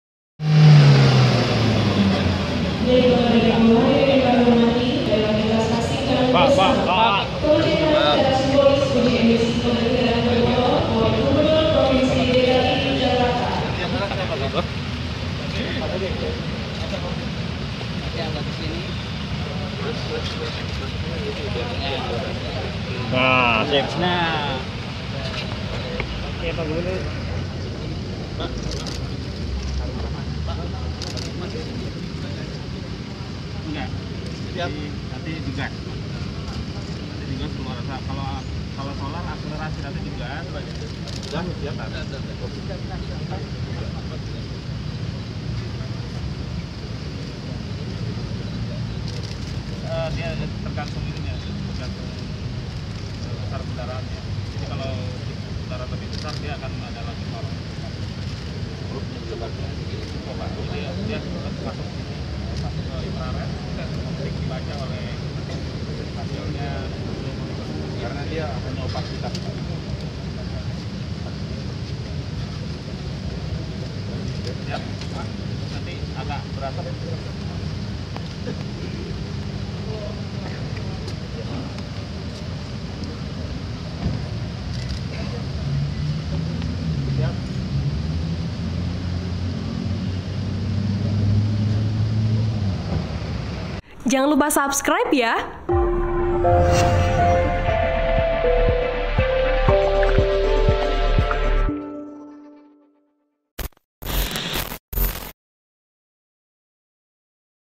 Anies Baswedan Merasa Senang Mobilnya Lulus Uji Emisi
Laporan Wartawan TribunJakarta.com, Muhammad Rizki Hidayat

TRIBUN-VIDEO.COM, GAMBIR - Gubernur DKI Jakarta, Anies Baswedan, merasa senang lantaran mobil pribadinya yang bermerek Toyota Kijang Innova hitam berpelat nomor B 1136 RFN, lulus uji emisi, di pendopo balai kota Jakarta, Selasa (13/8/2019).

"Bersyukur, selain diberi nikmat kesehatan, tadi mobil saya lulus uji emisi," kata Anies, pada acara Peresmian Aplikasi e-Uji Emisi dan Pengecekan Uji Emisi Kendaraan Bermotor, di halaman balai kota Jakarta.

Anies menyadari bahwa menjaga kualitas lingkungan hidup dimulai dari uji emisi kendaraan.

"Kita semua menyadari tantangan lingkungan hidup yang ada di seluruh dunia. Jakarta tidak berbeda menghadapi masalah yang sama," jelas Anies.(*)